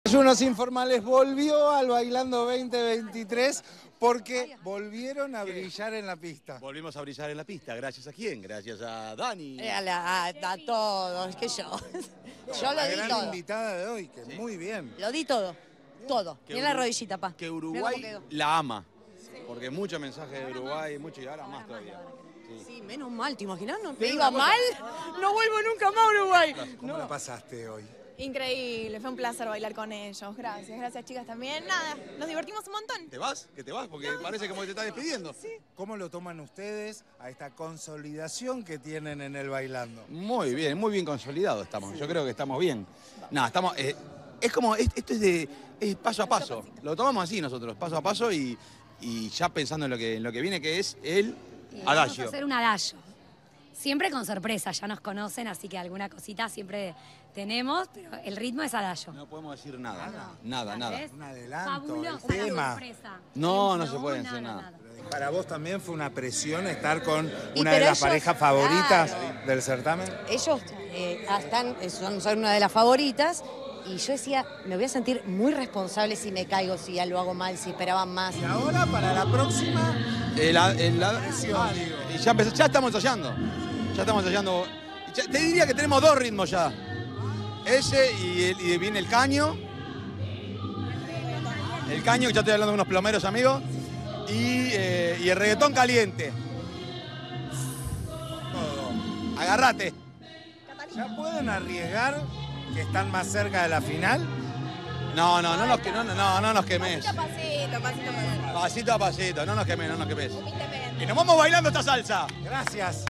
Maxi de la Cruz sobre Dani “La Chepi”: “Uruguay la ama”
La figura de La Tele volvió a sorprender con su baile en la salsa de a tres junto con Dani “La Chepi” y Cami Lonigro, y ya se prepara para el próximo ritmo, el adagio, que trae una nueva sorpresa.